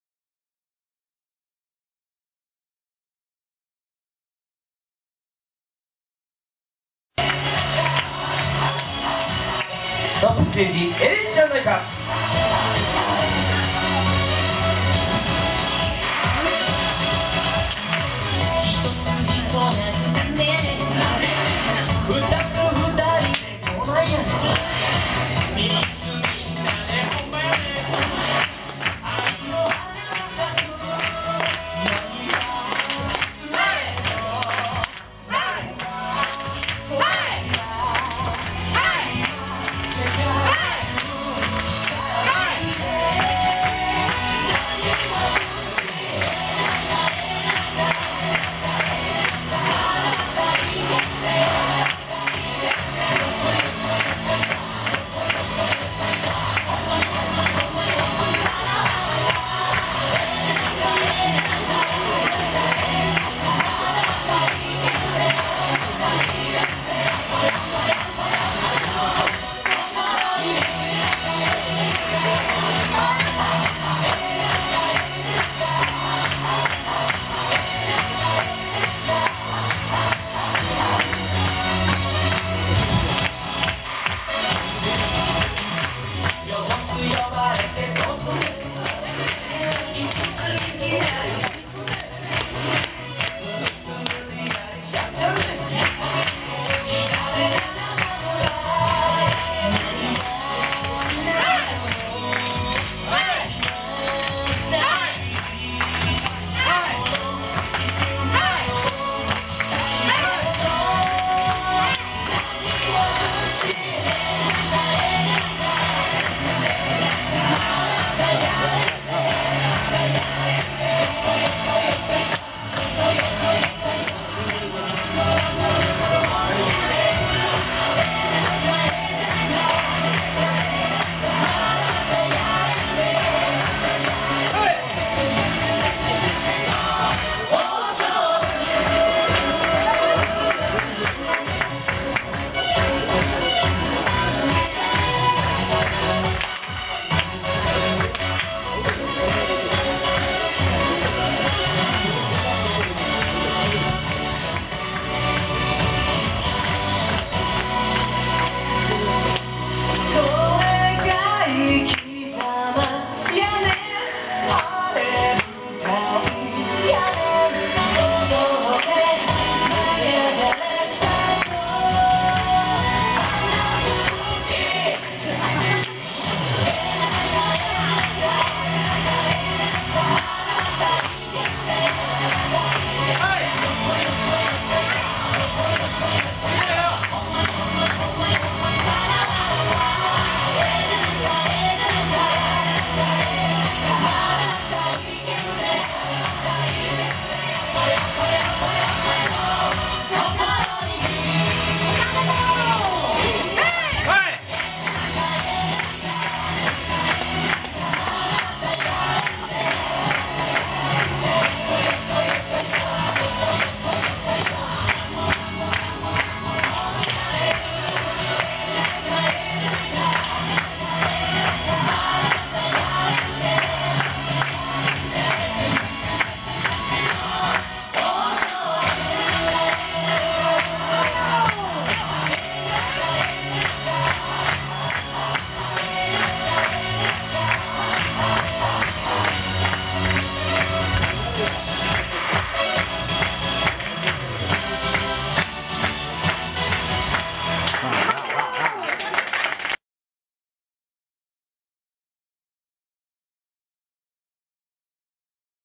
三河家劇団 ５・１ 幕開け 若さ爆発だ！
三河家桃太郎劇団　５・１　幕開け　ミニショーは、花形　京華太朗　中心に、、。
元気、元気でした。
バックで踊る、お二人の名前が、わかんなくて、、ごめんなさいね。
わあお！

H２７・５・１　梅南座、にて。